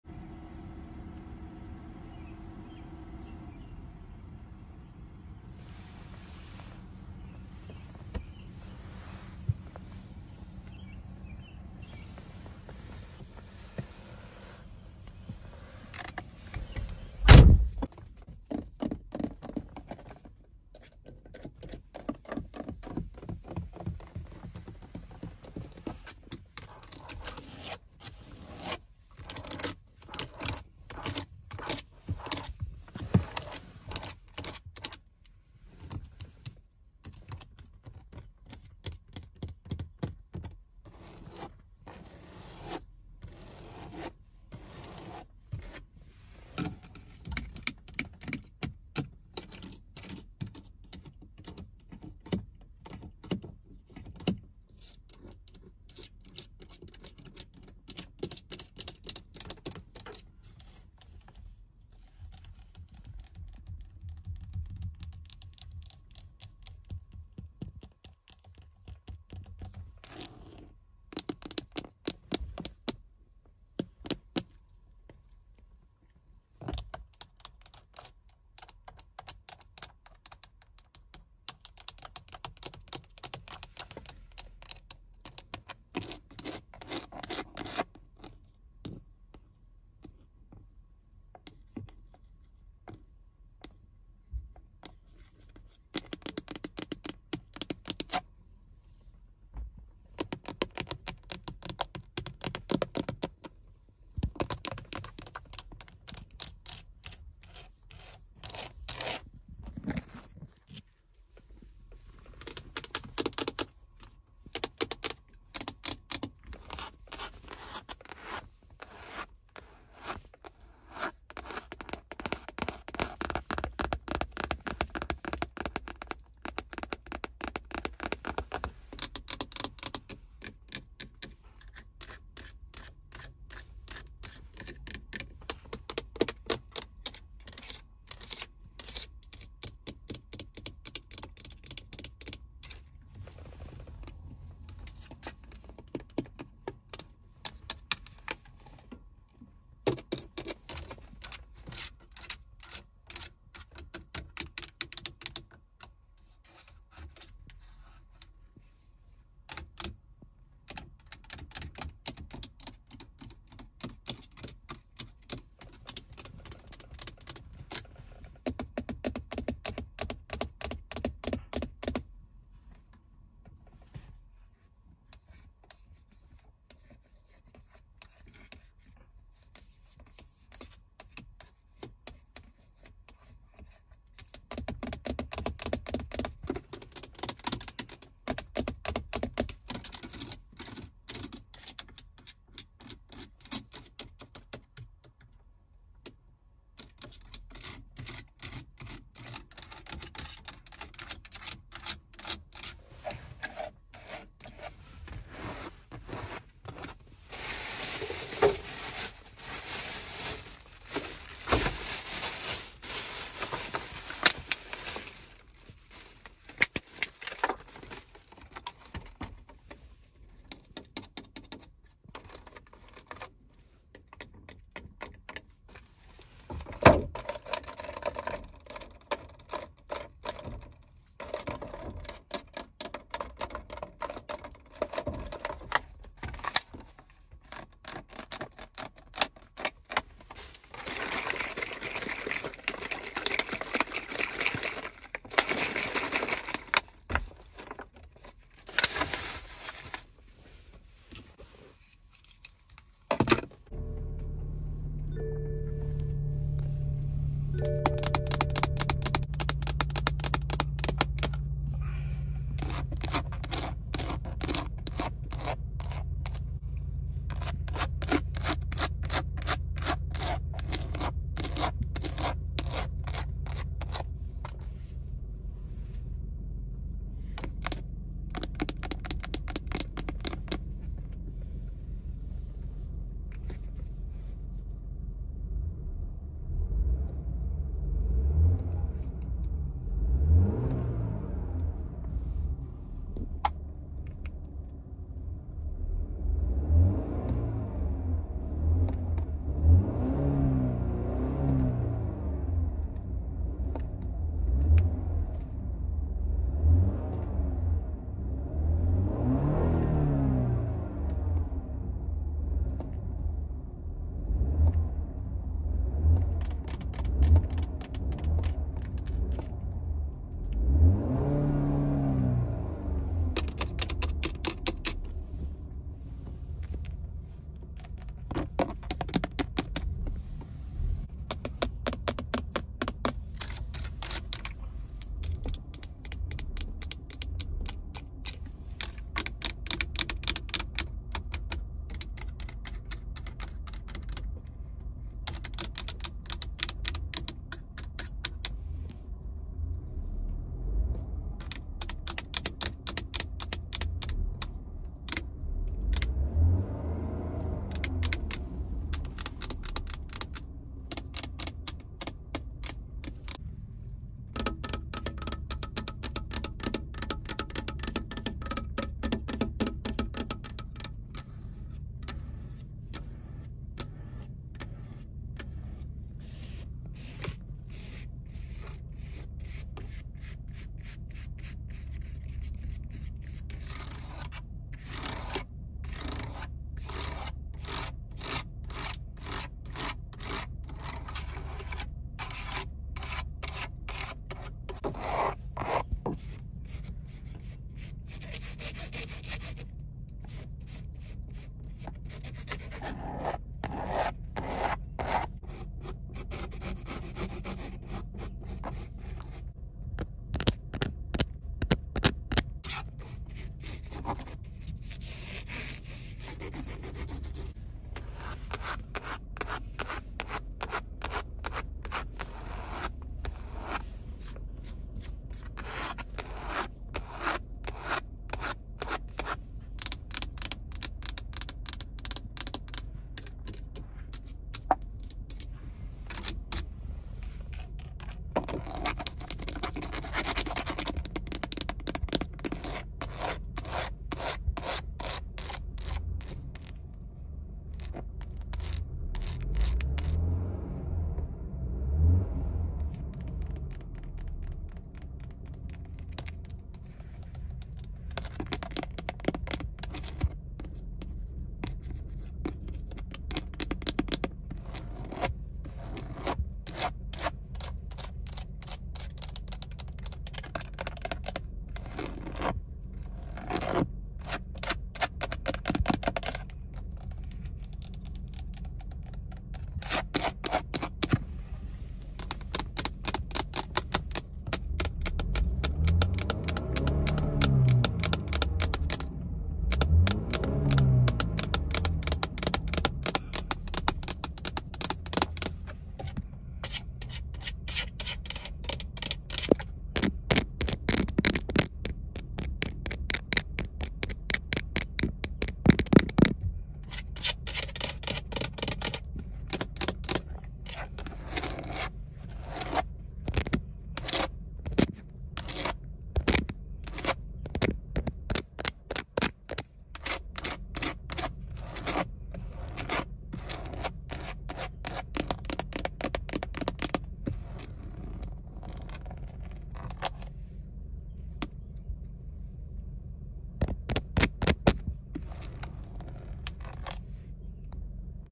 ASMR BMW REVVING, TAPPING & SCRATCHING SOUNDS | AMAZING SATISFYING SOUNDS FOR RELAXATION | #396
Enjoy the soft whispering, tapping and sounds to help you relax and rest easy.  Thanks for stopping by my lovelies. 🥰